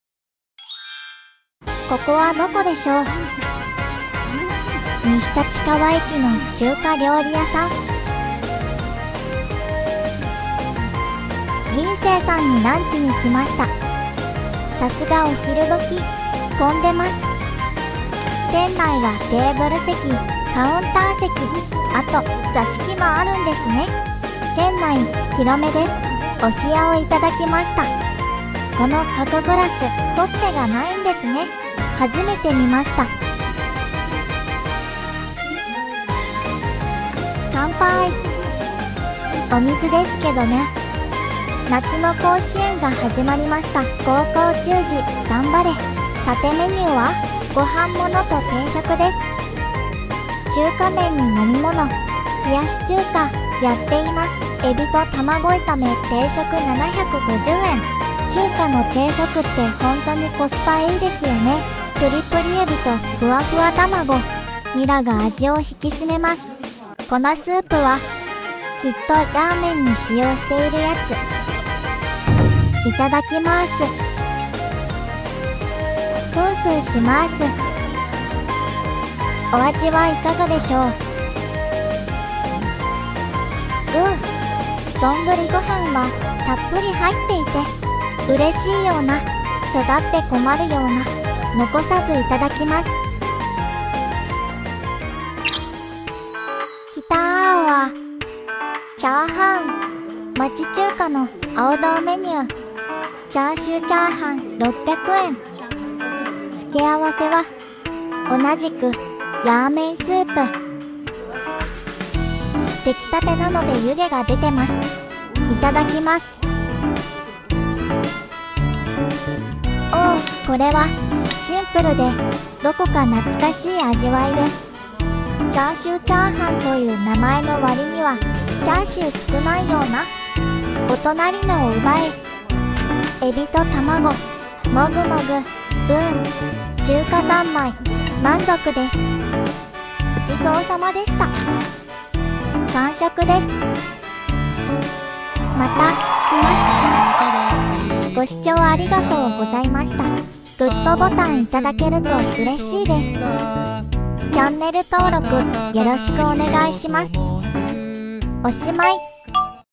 西立川の、町の中華屋さん【銀星】
【東京グルメ】国営昭和記念公園に行ったときの、オススメなお店です